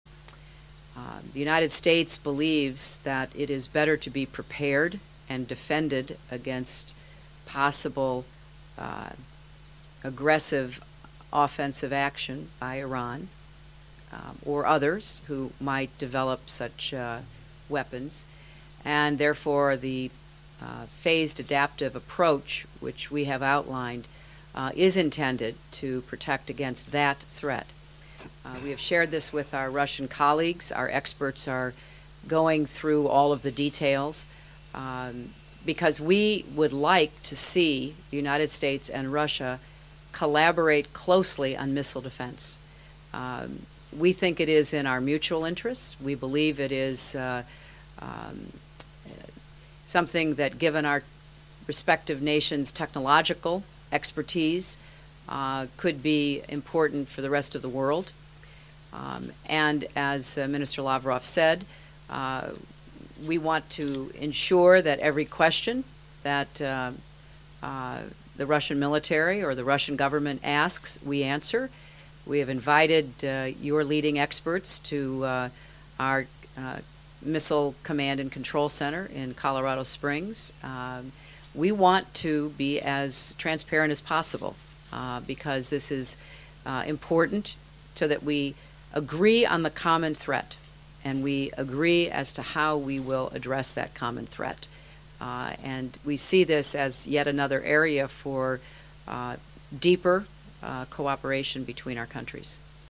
Clinton Urges Russian Support on Iran
Secretary of State Hillary Clinton met with Russian leaders in Moscow on Tuesday to urge their support in pressuring Iran to prove its nuclear program is peaceful. (Oct. 13)